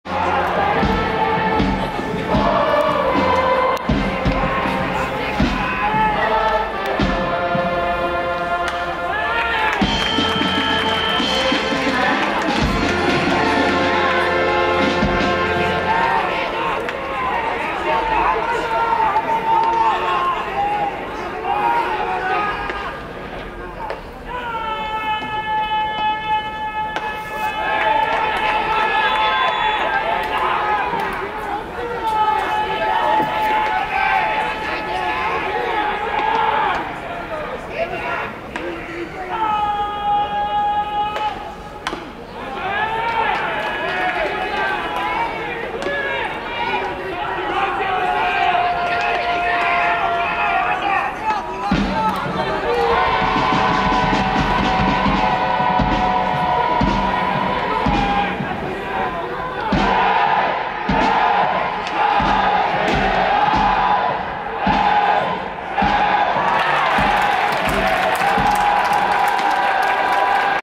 2022/10/19(水) 武内夏暉(國學院大学) vs 山下滉介(亜細亜大学) 空振り三振
東都大学野球 2022秋季リーグ戦(1部) 2022/10/19(水) 明治神宮野球場 亜細亜大学 vs 國學院大学 2回戦

7回裏（亜細亜大学の攻撃）
（先発投手）武内
0アウト 山下 空振り三振

【勝投手】武内
【敗投手】田中大

2022/10/8(土) 鈴木健(東京大学) vs 山田健太(立教大学) レフト2ランホームラン（打点2）
2022/8/31(水) 内海優太(広陵高校) 右中間ソロホームラン（打点1）打席(4回表) vs 篠木健太郎(法政大学)